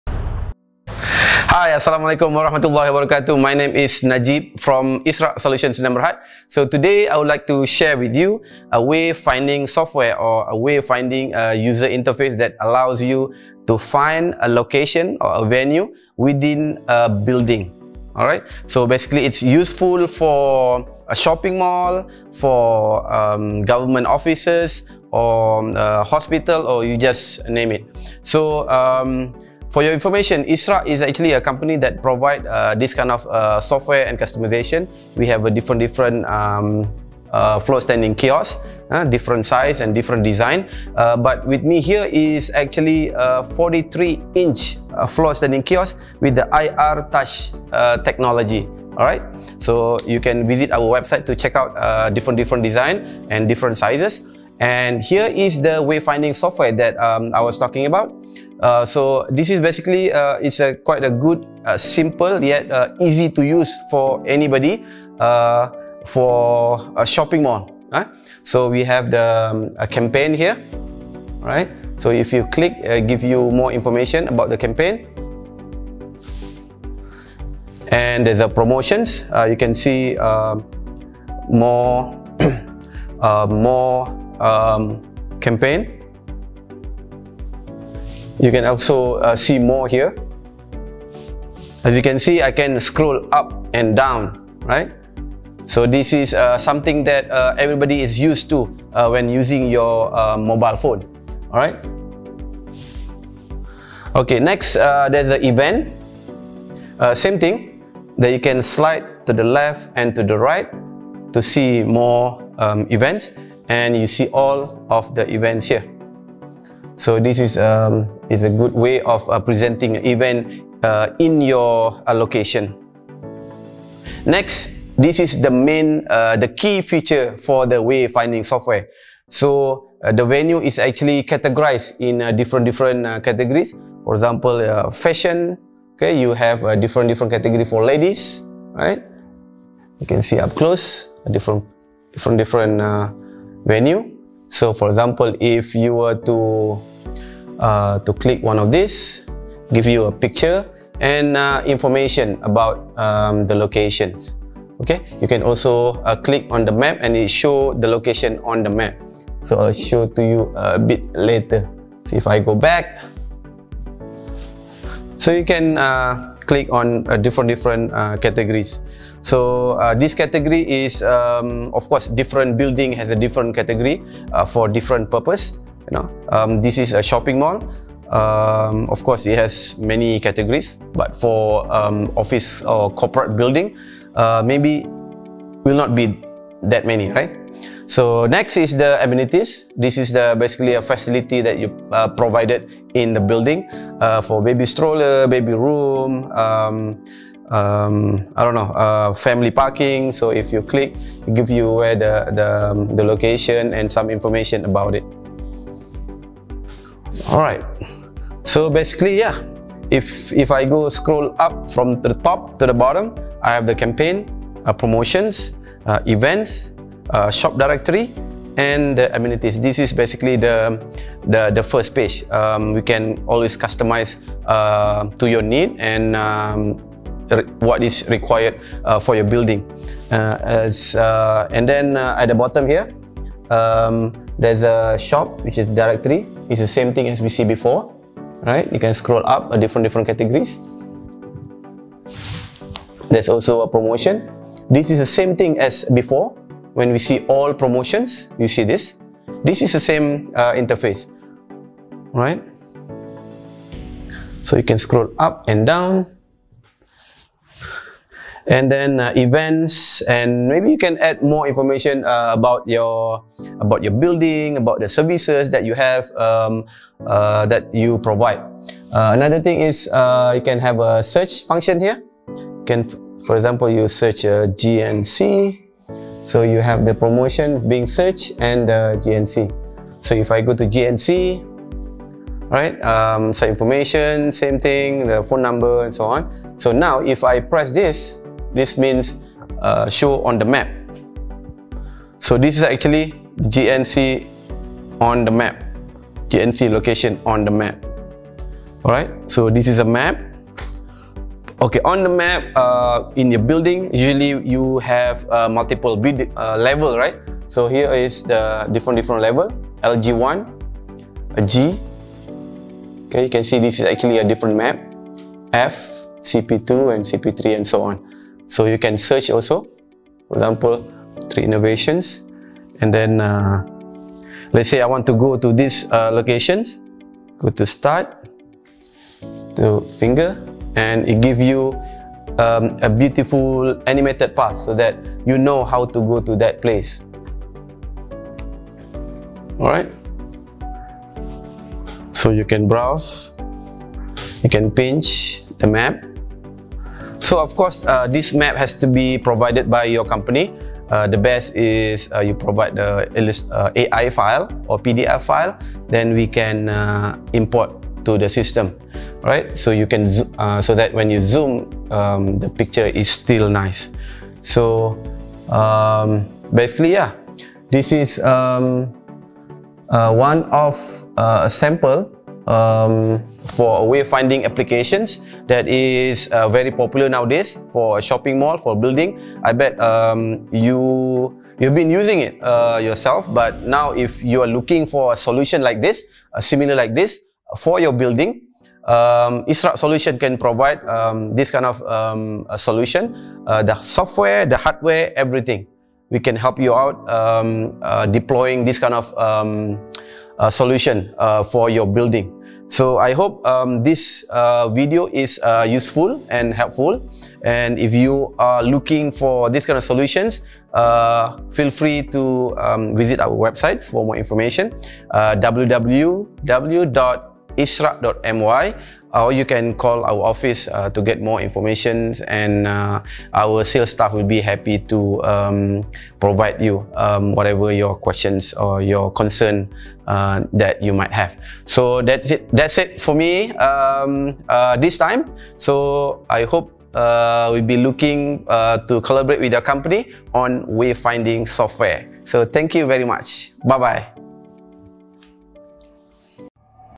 Interactive Wayfinding Software Kiosk 2020 for Shopping Mall, Offices, Hospital, Campus
Looking for an interactive touchscreen solution that allow user to easily and intuitively find their way around?

Arvia Digital Wayfinding Software,  it's suitable for corporate and college campuses, healthcare facilities, government buildings, hotel and conference centers and more. You will find our solution will be the perfect fit for your project. We have ready template for fast deployment or create custom design interactive wayfinding maps which lead your visitors through your facility and give your audience a convenient self-service source for information.

Our designers and creative team will work with your facility planners, marketing experts and project managers to concept, design, implement and test your interactive wayfinding touchscreen kiosks. Our solution is proven worldwide and now, we are offering to any organization the best-in-class Digital Wayfinding Solution in Malaysia.